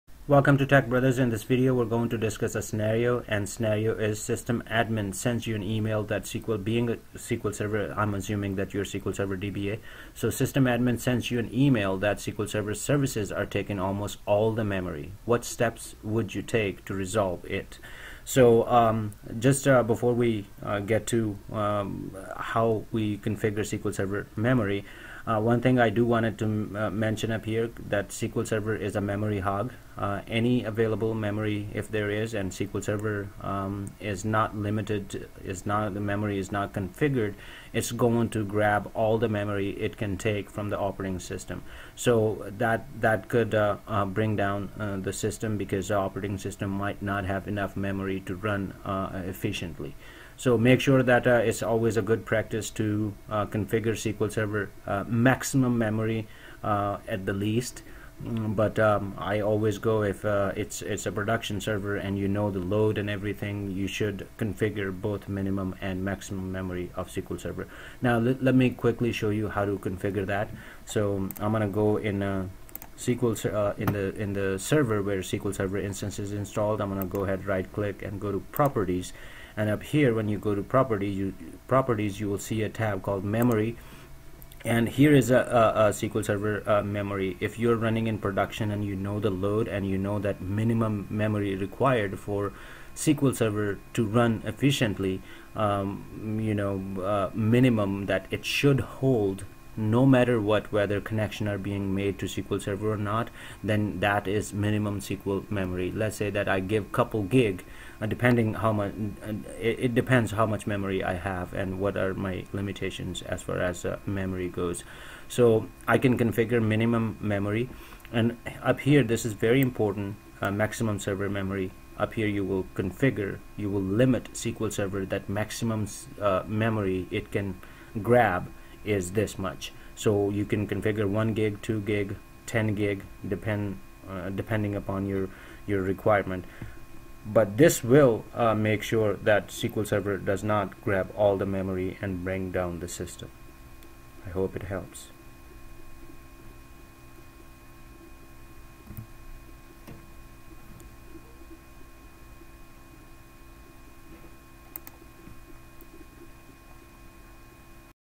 SQL Server DBA Interview | How to resolve Memory issues related to SQL Server Instance
SQL Server DBA Interview question "System Admin sends you an email that SQL Server services are taking almost all the memory, what steps would you take to resolve it?"